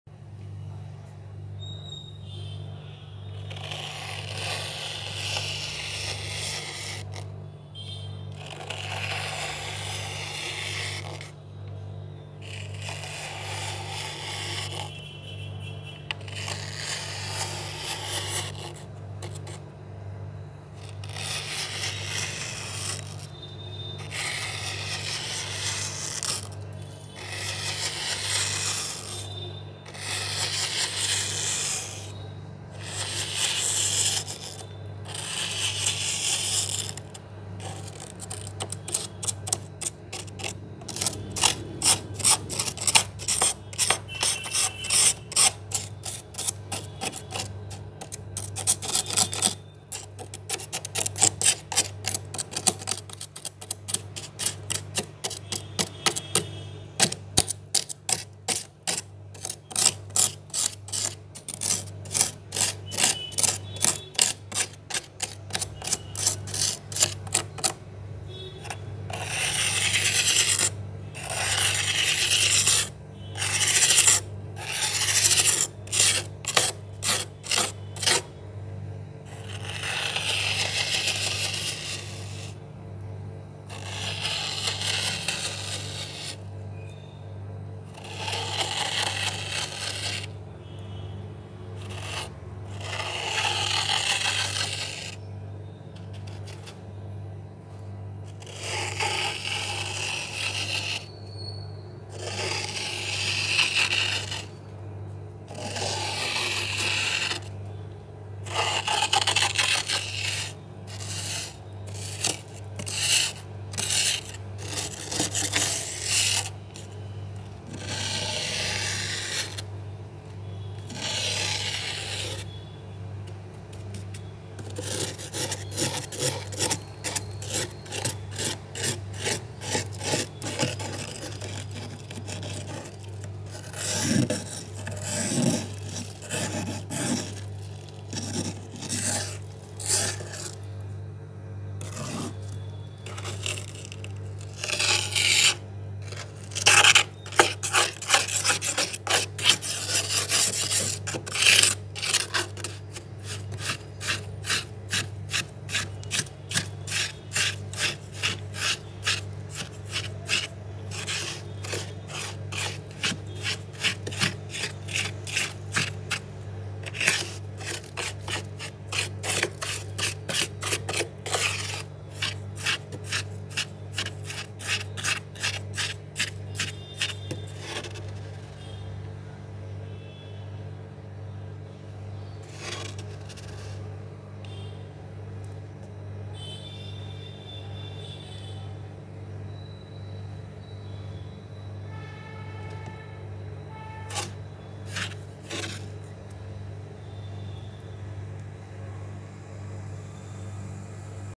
Asmr shiny freezer frost|| scraping with spoon🤩🤩|| satisfying sounds 🤤🤤🤤
Ice madness asmr:-)